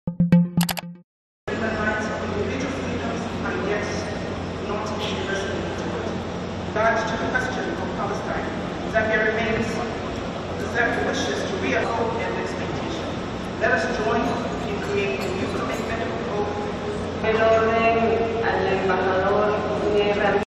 Plenaria sobre paz y soberanía reanudada por cancilleres del MNOAL
Isla Margarita (Venezuela), 16 sep (EFE), (Imagen: Jackdwin Sáez ).- Los ministros de Exteriores del Movimiento de países No Alineados (MNOAL) reanudaron hoy sus conversaciones sobre paz, soberanía y seguridad en una plenaria que comenzó ayer en el marco de la XVII cumbre del foro que se celebra en la isla venezolana de Margarita.